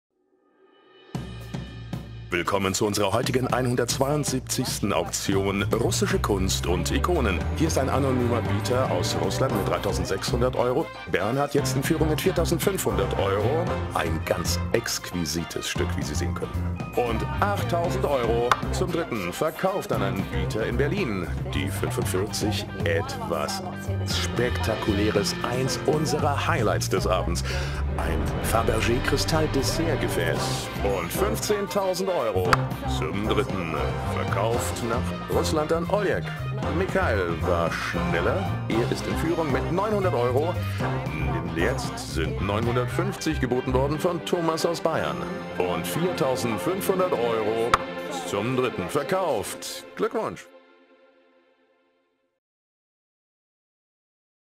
Auctionata Auction Highlights: No. 172 Russian Works of Art & Icons
The fine quality of Russian art was aptly demonstrated by this exciting auction, which bore witness to the union of masterful artistic craftsmanship with superior materials. A range of masterpieces went under the hammer, including icons, finely crafted silverware and other impressive collectors’ items. View the auction results